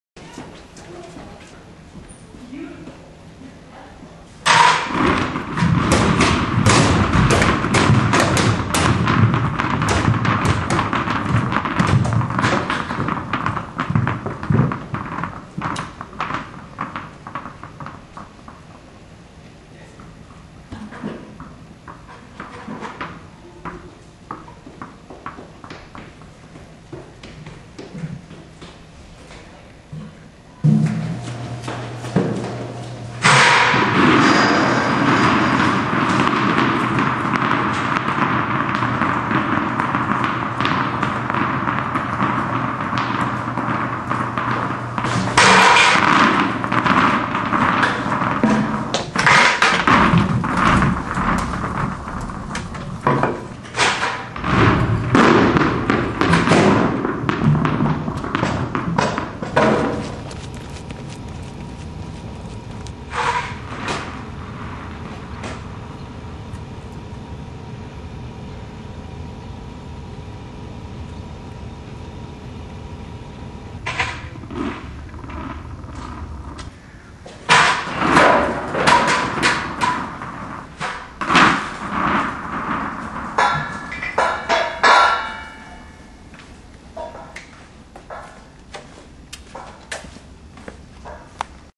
Making of Tennis Balls
Original footage when I was making tennis balls.